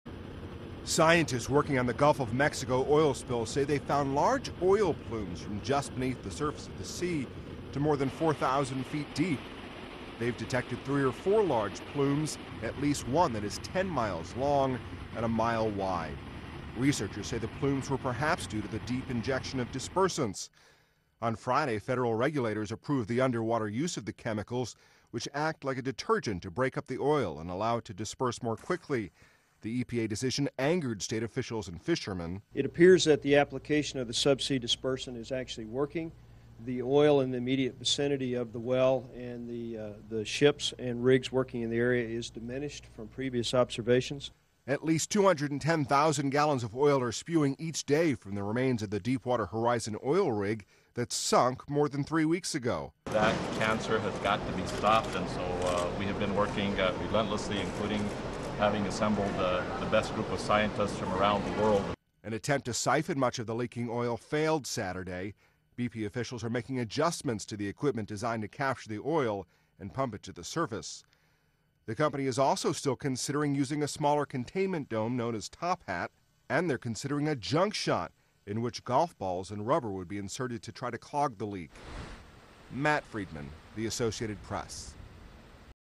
Giant Oil 'Plumes' Detected in Gulf of Mexico
Scientists have found huge plumes of oil lurking under the surface of the water in the Gulf of Mexico, as BP hit a snag in its latest effort to slow down the oil blasting out of a broken undersea pipe. (May 16)